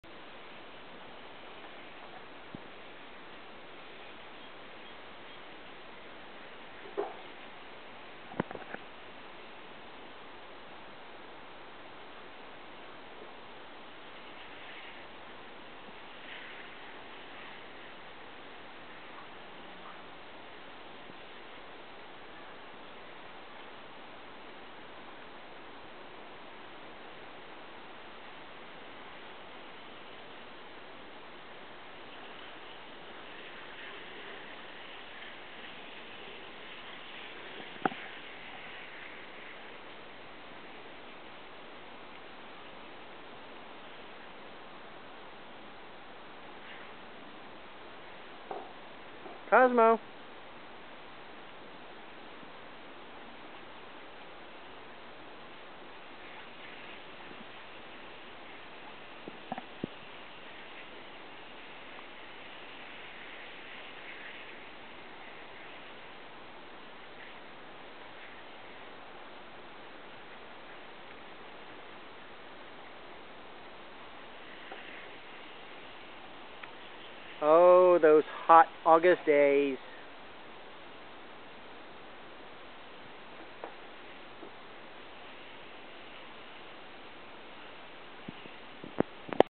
English Bulldog Getting cooled off from the Texas sun!!
My Bulldog, Cozmo, "loves" the sprinkler and cooling off from the Hot, Texas Summer Sun!! He likes a nice pool but the low pressure wash is what he Really Loves.